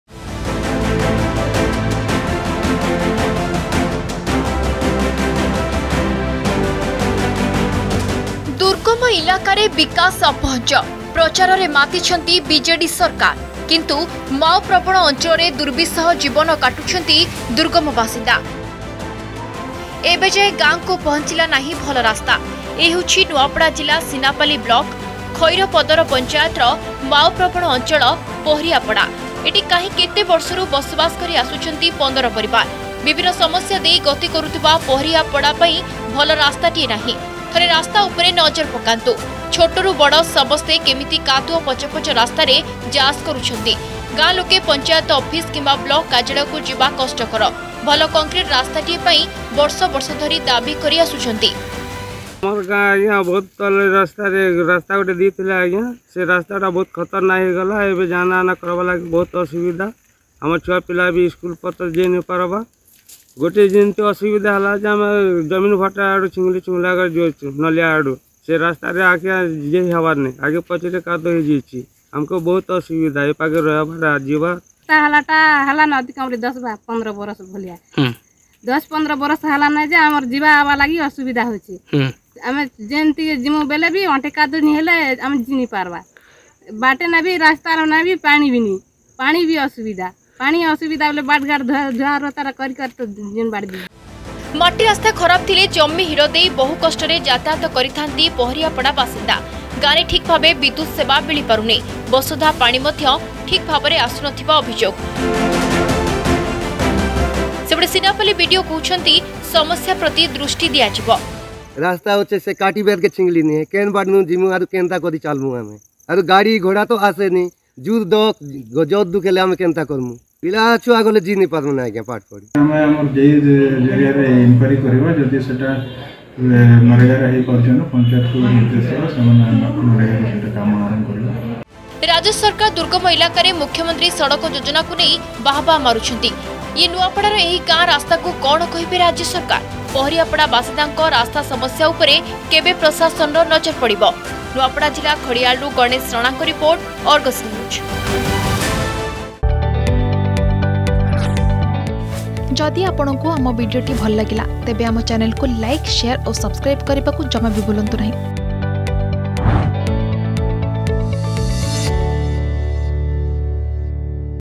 Paharipada In Nuapada District Is Looking For Development
People are living cursed life. Paharipada in Nuapada district is looking for development. Govt schemes are a dream in Khairapadar region under Sinapali block of Nuapada l